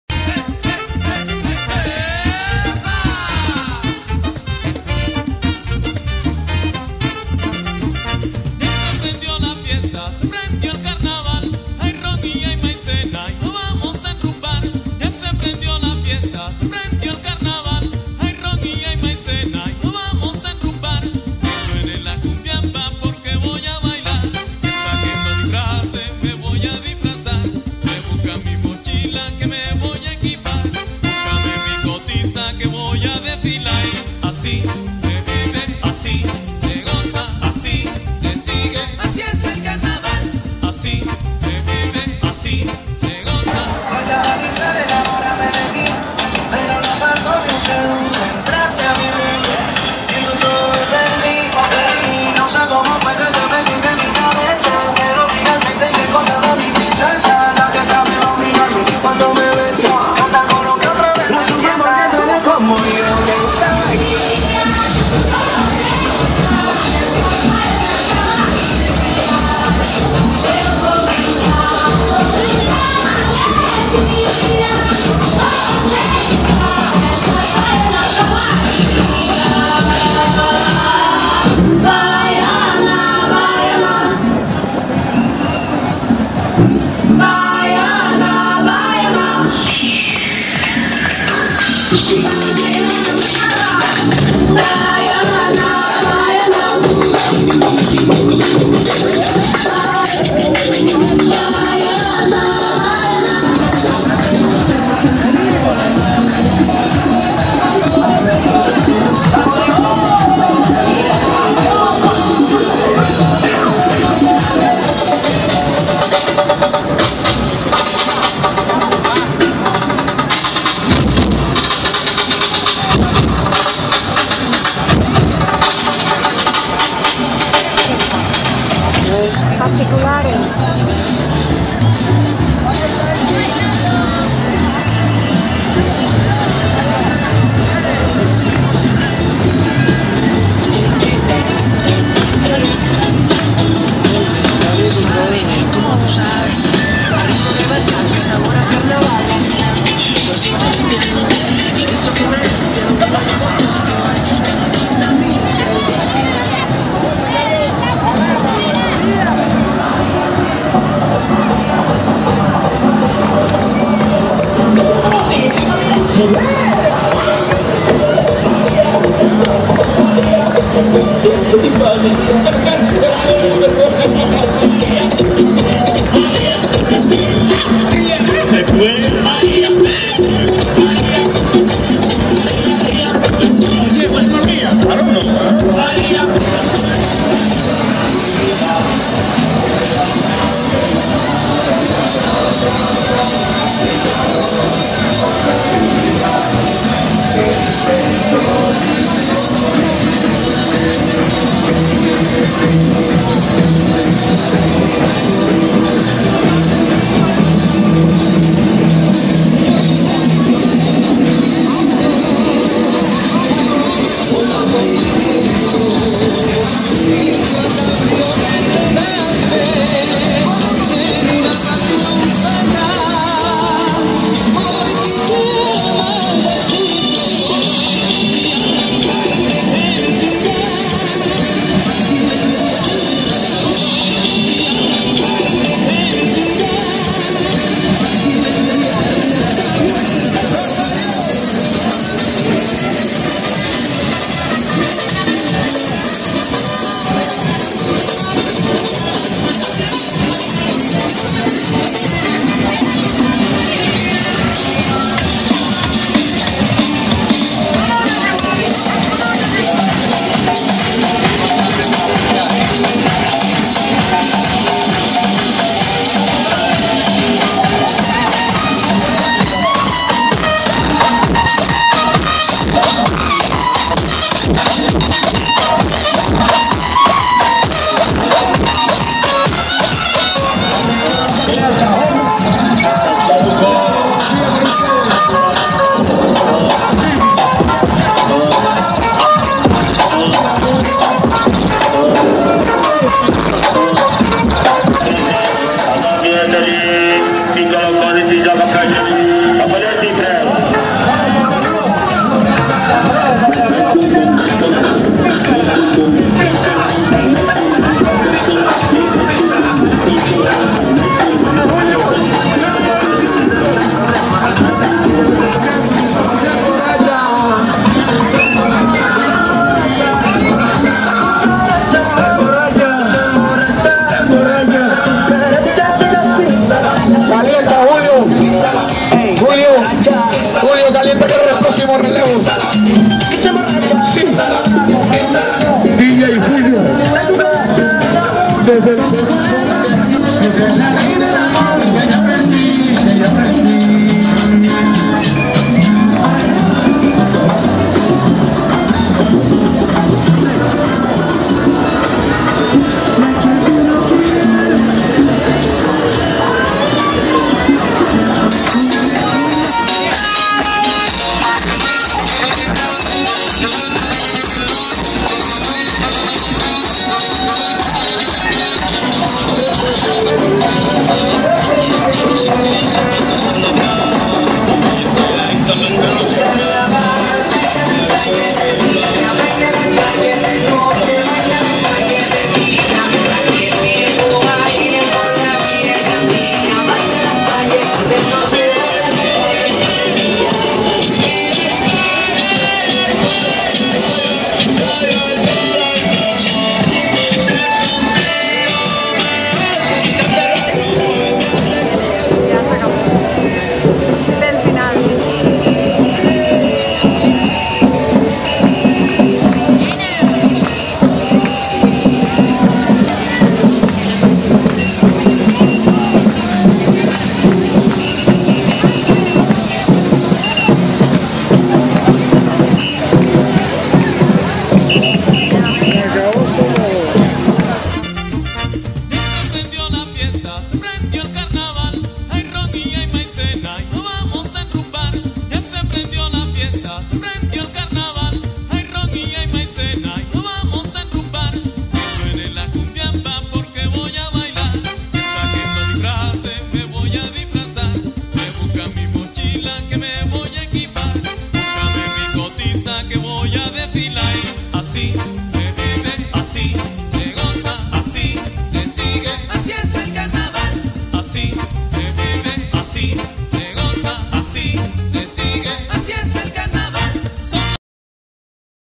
Carnaval de Tenerife 2020. El Coso. Parte VIII
Cierre del Desfile del Coso 2020, con las carrozas  representando a diversos Comercios, Bares, Industrias y Otros.. Muy bonito y animado este desfile carnavalero donde los niños, familias y turistas se unen  en esta zona  para disfrutarlo...
Vídeo realizado INSITU durante el desfile del Coso  del Carnaval de Tenerife 2020, las músicas que se escuchan durante el desfile la traían las carrozas que participaron
En este video no aparecen niños